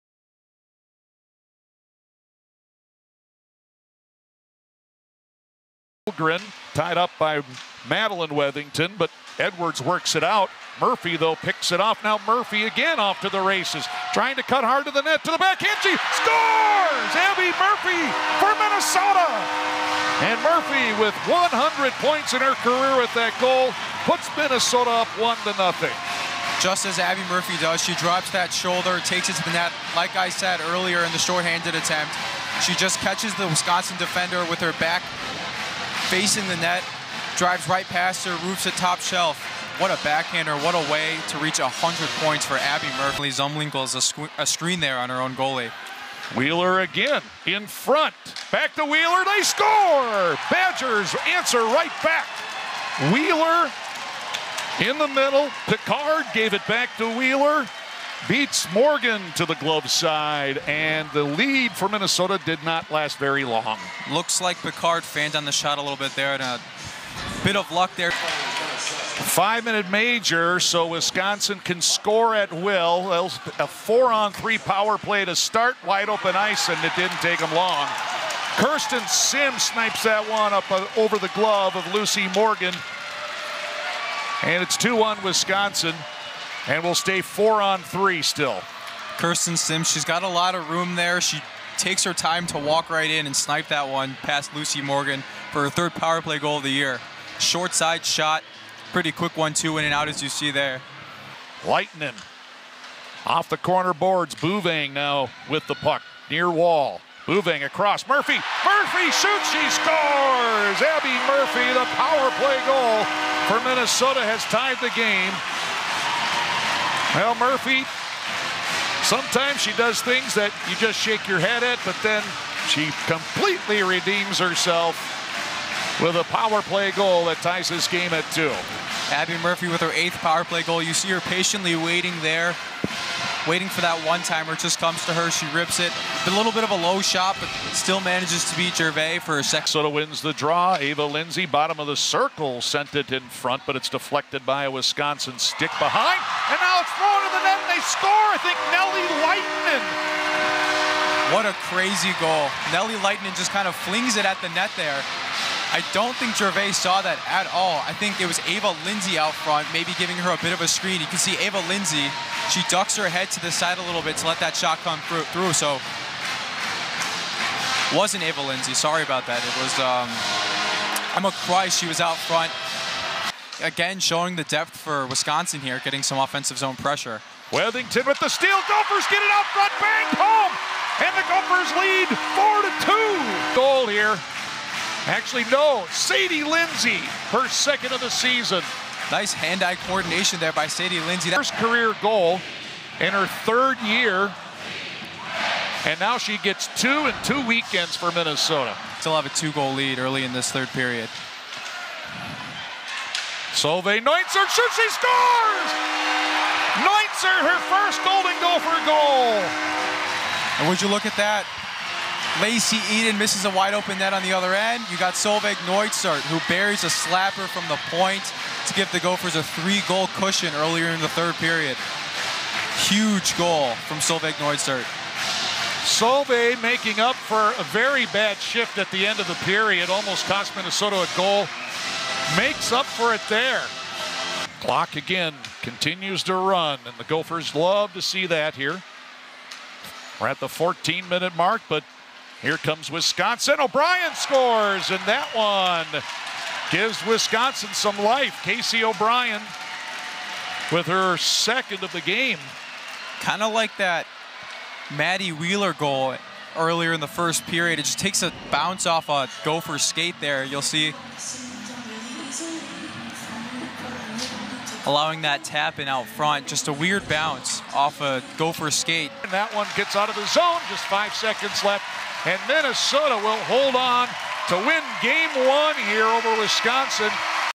Wisconsin vs Minnesota Women’s College Hockey Highlights December 8, 2023
These two will be playing each other this Friday for the WCHA Final Faceoff! (which I will be doing FULL highlights for)

With World Championships just a month away I thought I’d highlight college players from each team who have played for their country’s national team

MINNESOTA GOLDEN GOPHERS
#7 Nelli Laitinen FIN
#10 Emily Zumwinkle USA
#11 Josefin Bouveng SWE
#13 Emma Kreisz HUNGARY
#27 Solvieg Neunzert SWE
#28 Abbey Murphy USA

WISCONSIN BADGERS:
#4 Caroline Harvey USA
#5 Anna Wilgren USA
#6 Lacey Eden USA
#10 Laila Edwards USA
#17 Britta Curl USA
#26 Casey O’Brian USA
#27 Kirsten Simms USA

This is their first matchup from December 8, 2023 in Minnesota.